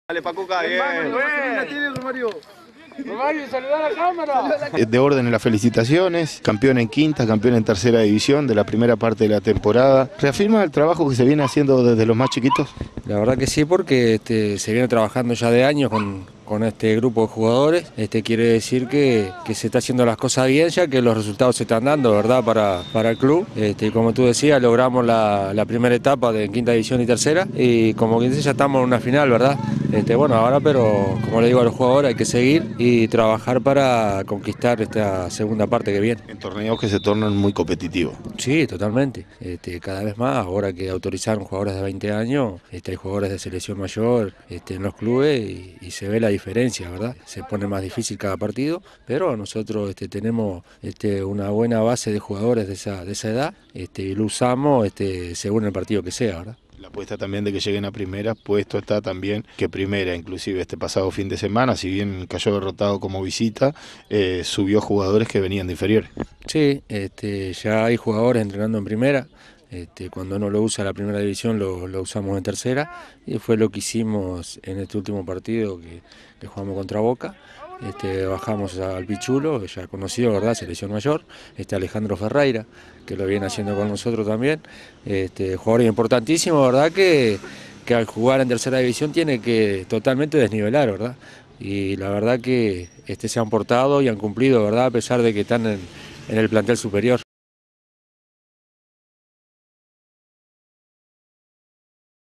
FORMATIVAS
FORMATIVAS – En Quinta y Tercera,  Melo Wanderers bajo la batuta de julio Rosa se adjudico los torneos Apertura